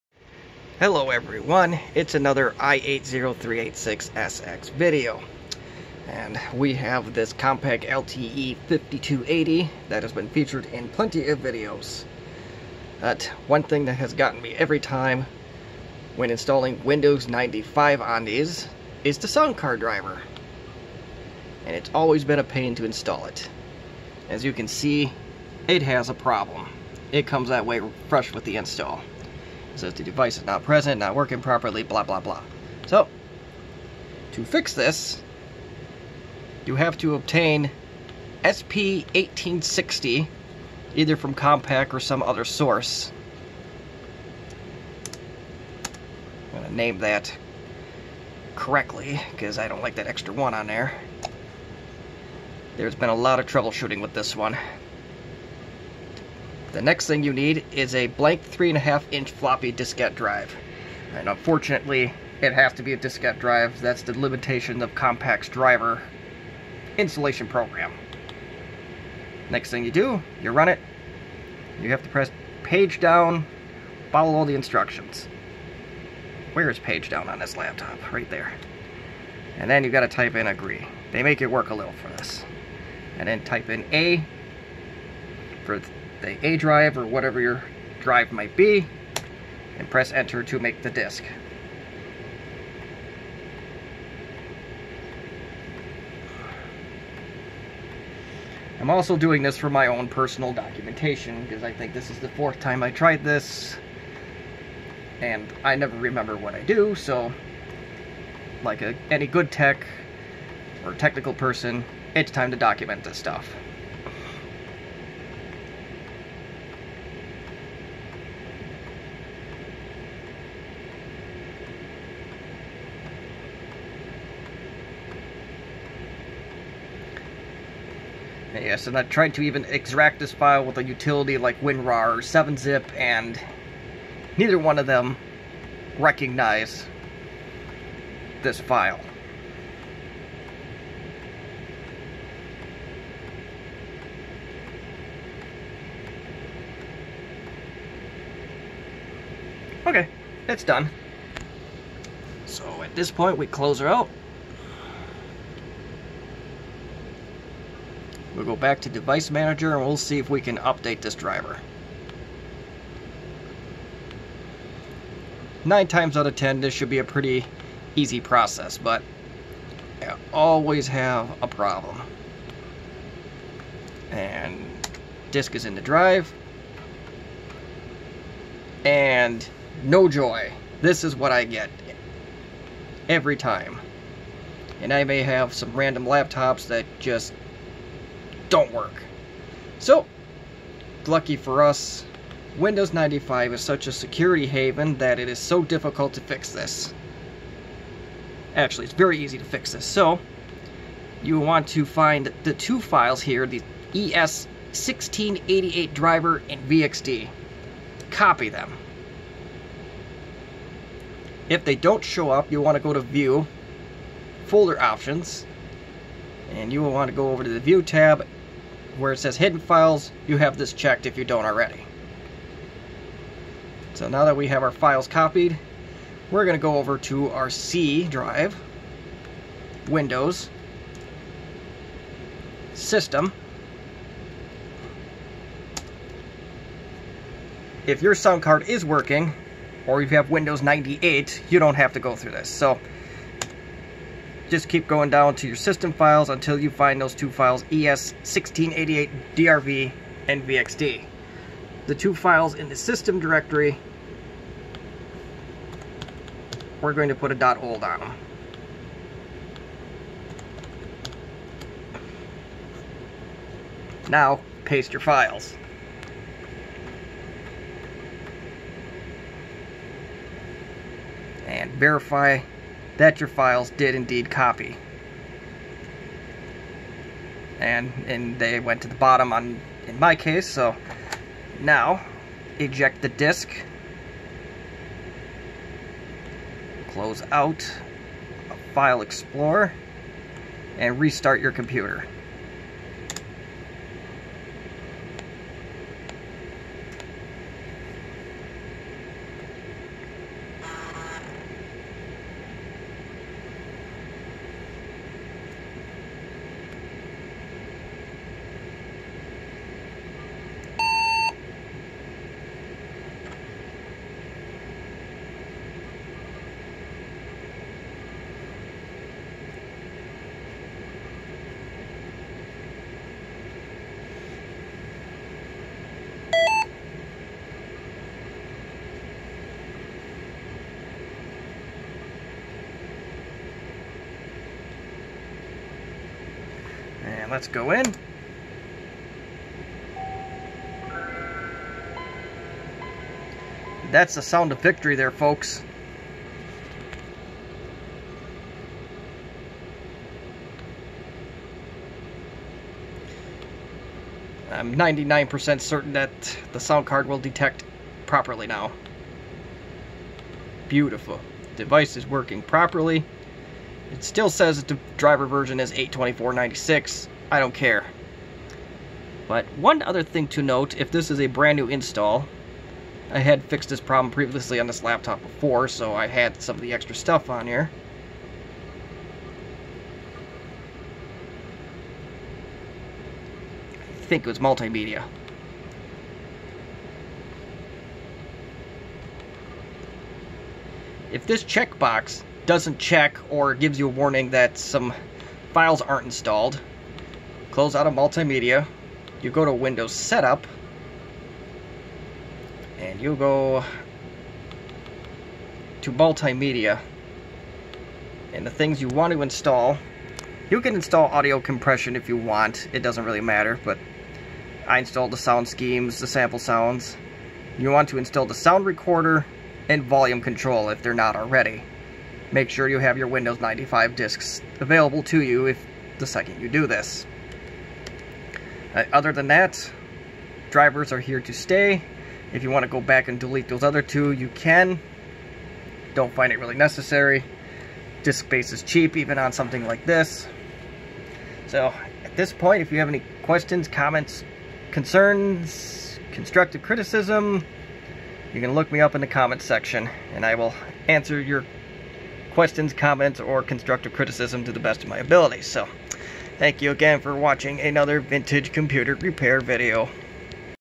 Fix sound issue on Compaq LTE 5000 series running Windows 95
This video will show you how to get your sound card working properly on a Compaq LTE 5000 series running Windows 95.  You will need sp1860.exe and a diskette drive to do this.  SP1860.exe is the driver from HP/Compaq.  I may make a custom installer for this if there is enough demand.  Typically Windows will find the driver directly from the disk, but I had no luck with this sound card and driver combination.

Important:I make no guarantees that this will work for you.  You may have a software or hardware problem I don't know about.  This process is for Windows 95 only.  Windows 98 and ME detect the sound card properly.

The computer used is my Compaq LTE 5280.